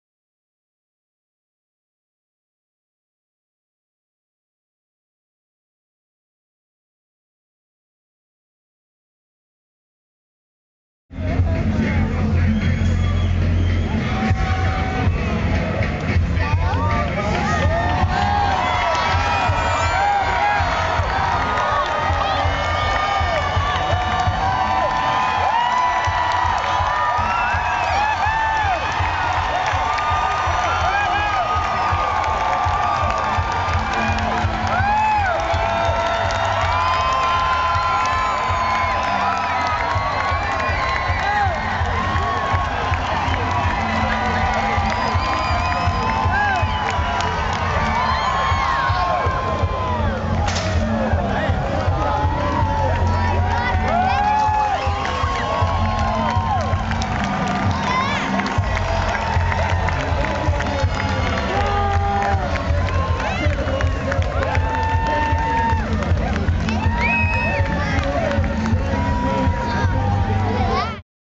Tall bike ride, attempt #2 at 2008 LAAFF
Here's the second attempt of bicycle rider Michael Mooney to set a Guiness World Record by riding his 44-foot-tall bicycle at least 100 meters at the 2008 Lexington Avenue Arts and Fun Festival on Sept. 7, 2008.